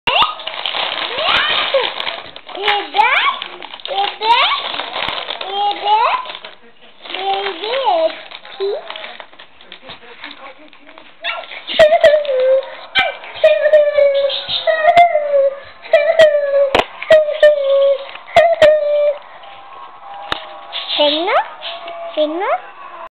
Riley playing with his peas
Empties his dorito bag full of toys, drops a pea...sad, sad, pea!